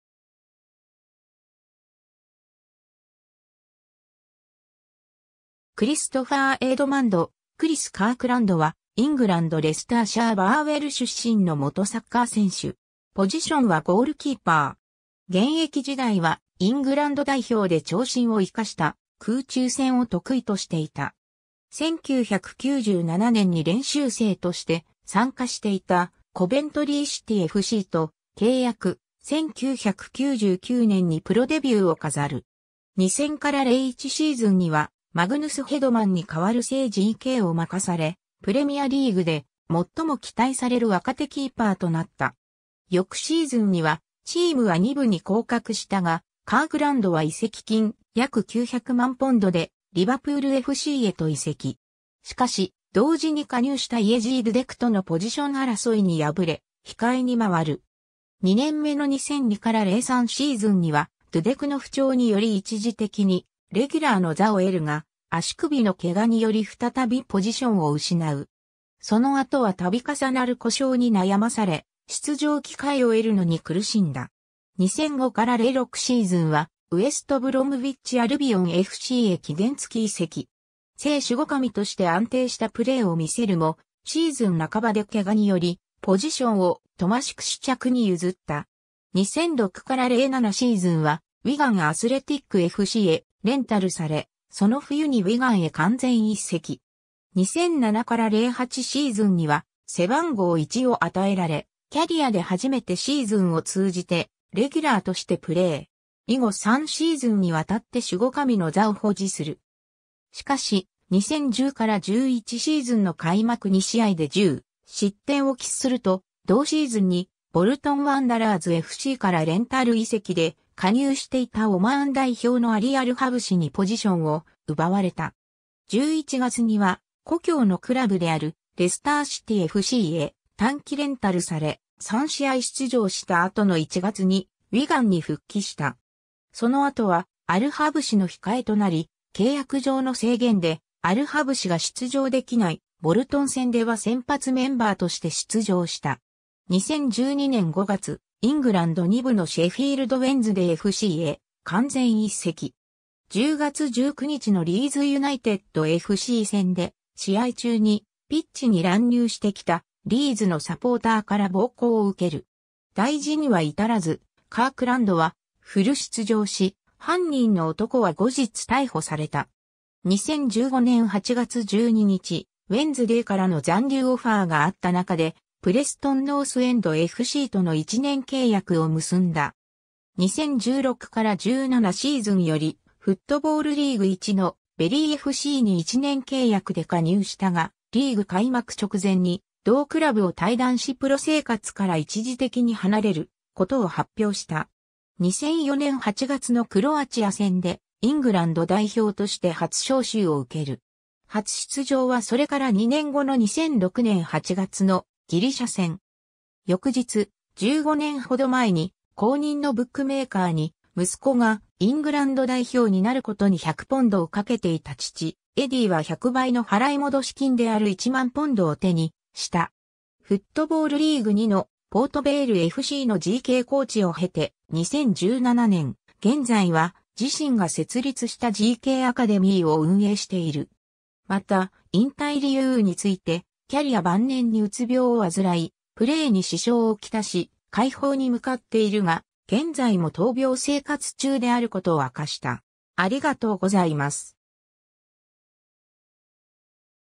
クリス・カークランド
コヴェントリー・シティFCの選手
ウィガン・アスレティックFCの選手
レスター・シティFCの選手
ドンカスター・ローヴァーズFCの選手
シェフィールド・ウェンズデイFCの選手
プレストン・ノースエンドFCの選手
クリストファー・エドマンド・"クリス"・カークランド（Christopher Edmund "Chris" Kirkland, 1981年5月2日 - ）は、イングランド・レスターシャー・バーウェル出身の元サッカー選手。
 現役時代は、イングランド代表で長身を生かした空中戦を得意としていた。
 1997年に練習生として参加していたコヴェントリー・シティFCと契約、1999年にプロ・デビューを飾る。
2000-01シーズンにはマグヌス・ヘドマンに代わる正GKを任され、プレミアリーグで最も期待される若手キーパーとなった。
 翌シーズンにはチームは2部（フットボールリーグ・チャンピオンシップ）に降格したが、カークランドは移籍金約900万ポンドでリヴァプールFCへと移籍。
しかし、同時に加入したイェジー・ドゥデクとのポジション争いに敗れ、控えにまわる。
2年目の2002－03シーズンにはドゥデクの不調により一時的にレギュラーの座を得るが、足首の怪我により再びポジションを失う。
その後は度重なる故障に悩まされ出場機会を得るのに苦しんだ。
 2005-06シーズンはウェスト・ブロムウィッチ・アルビオンFCへ期限付き移籍。
正守護神として安定したプレーをみせるも、シーズン半ばで怪我によりポジションをトマシュ・クシュチャクに譲った。
 2006-07シーズンはウィガン・アスレティックFCへレンタルされ、その冬にウィガンへ完全移籍。
2007-08シーズンには背番号1を与えられ、キャリアで初めてシーズンを通じてレギュラーとしてプレー。
以後3シーズンに渡って守護神の座を保持する。
しかし、2010-11シーズンの開幕2試合で10失点を喫すると、同シーズンにボルトン・ワンダラーズFCからレンタル移籍で加入していたオマーン代表のアリ・アル・ハブシにポジションを奪われた。
11月には故郷のクラブであるレスター・シティFCへ短期レンタルされ、3試合出場した後の1月にウィガンに復帰した。
その後はアル・ハブシの控えとなり、契約上の制限でアル・ハブシが出場できないボルトン戦では先発メンバーとして出場した。
 2012年5月、イングランド2部のシェフィールド・ウェンズデイFCへ完全移籍。
10月19日のリーズ・ユナイテッドFC戦で試合中にピッチに乱入してきたリーズのサポーターから暴行を受ける。
大事には至らずカークランドはフル出場し、犯人の男は後日逮捕された。
 2015年8月12日、ウェンズデイからの残留オファーがあった中でプレストン・ノースエンドFCとの1年契約を結んだ。
 2016-17シーズンよりフットボールリーグ1のベリーFCに1年契約で加入したが、リーグ開幕直前に同クラブを退団しプロ生活から一時的に離れることを発表した（事実上の現役引退）。
 2004年8月のクロアチア戦でイングランド代表として初招集を受ける。
初出場はそれから2年後の2006年8月のギリシャ戦。
翌日、15年ほど前に公認のブックメーカーに「息子がイングランド代表になる」ことに100ポンドを賭けていた父エディーは100倍の払い戻し金である1万ポンドを手にした。
 フットボールリーグ2のポート・ヴェイルFCのGKコーチを経て、2017年現在は自身が設立したGKアカデミーを運営している。
また引退理由について、キャリア晩年にうつ病を患い、プレーに支障をきたし、快方に向かっているが現在も闘病生活中であることを明かした。